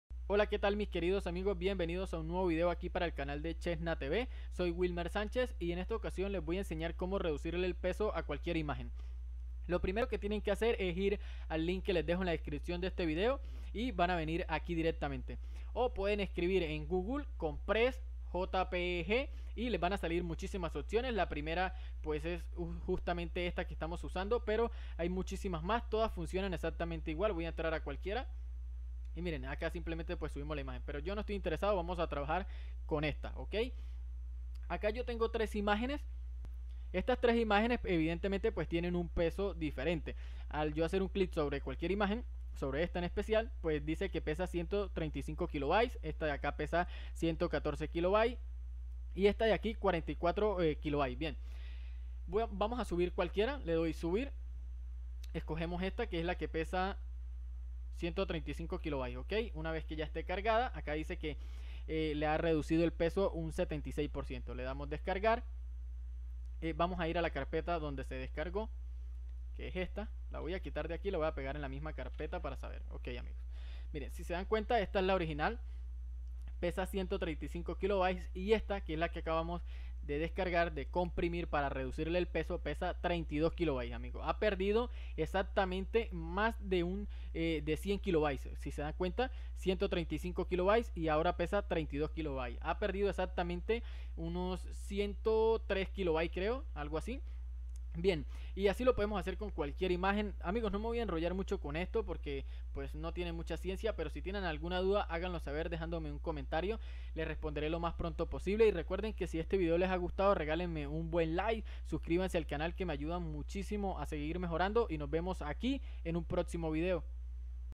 Cómo REDUCIR el peso de IMÁGENES Hasta un 90%⏪ | Sin Programas ✅ / Chezna TV
en este video les explico como bajar el peso de imágenes, fotográficas etc. de una forma facil y rapida sin instalar programas. no tardaras ni 15 segundos en hacerlo